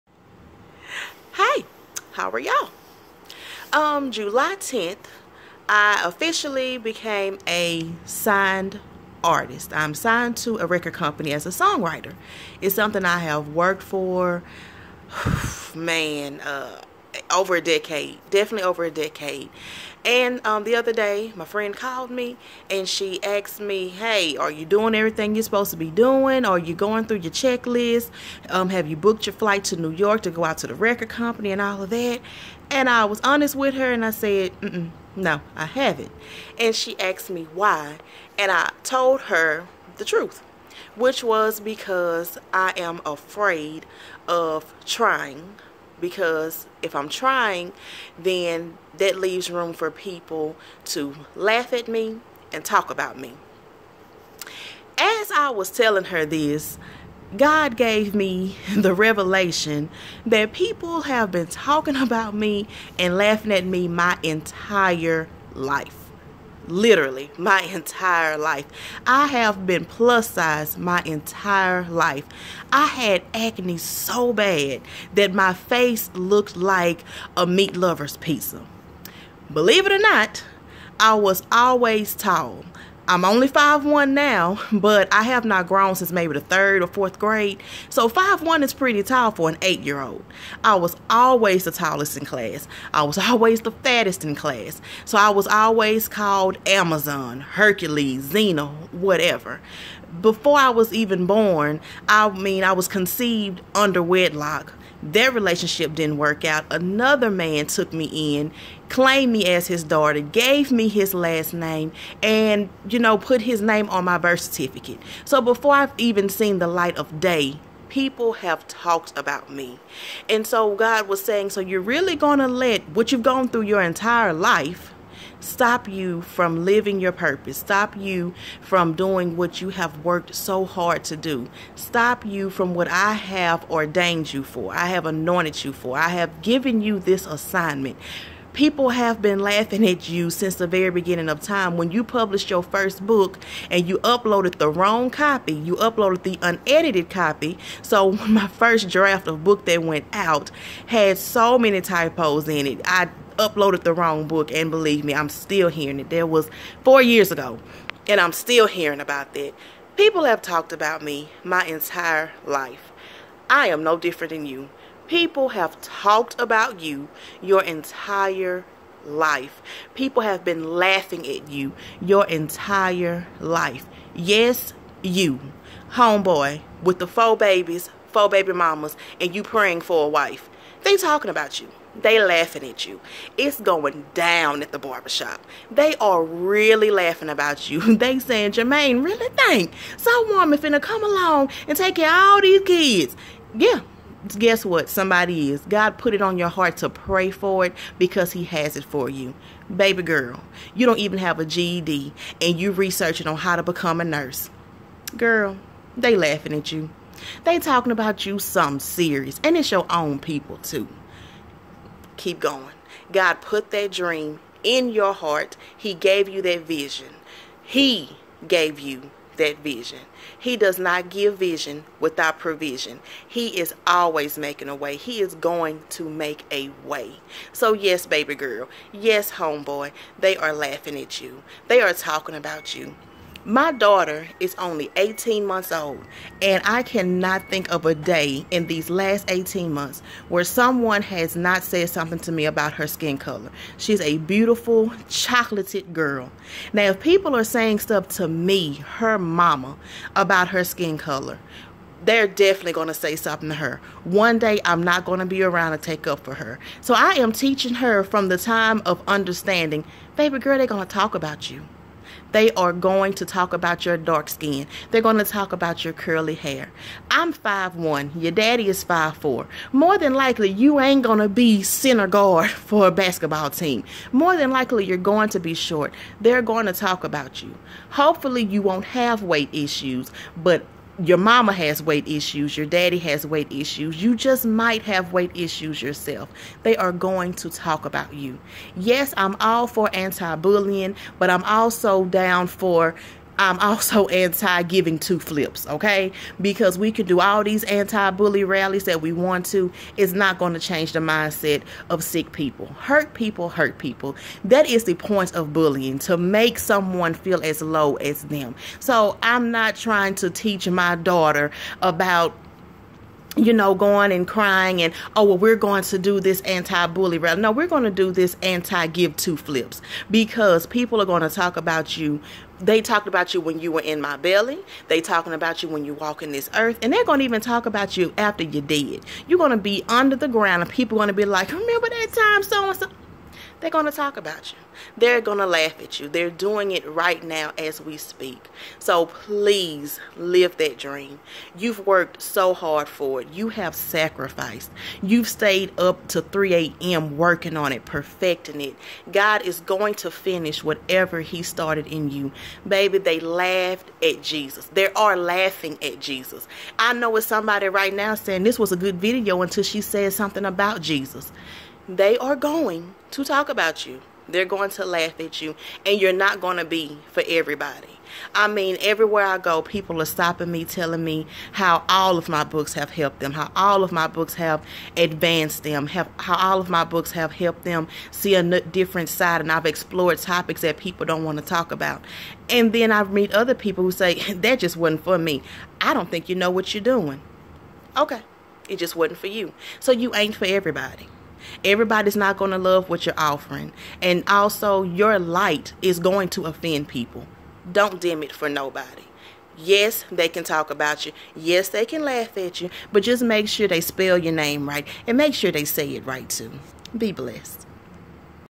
People are Talking About You
Keep living and pursuing your dream despite what people have to say.